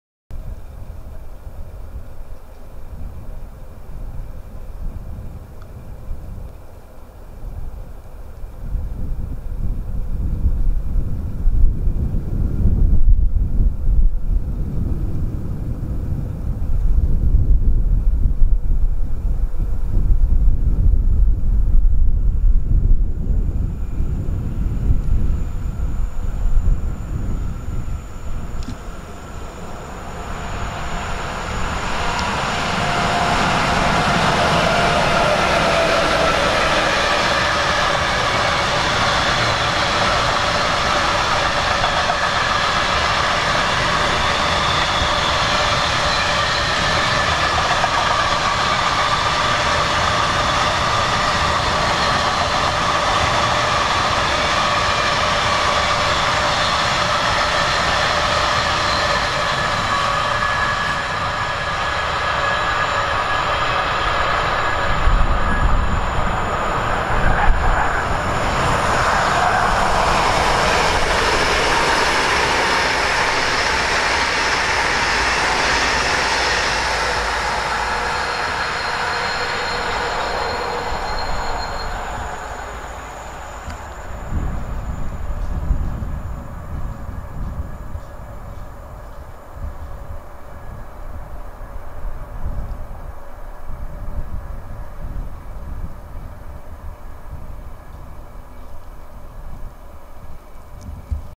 E483-224 CD Cargo z węglarkami VTG mija ED250-007 z EIP 5304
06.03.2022
Bardzo zimny i wietrzny dzień, temperatura oscylująca w okolicach 1 stopnia Celsjusza. nagrania pochodzą z linii kolejowej numer 9 w okolicach wsi Turza Wielka. Zostały wykonane aparatem panasonic lumix fz300.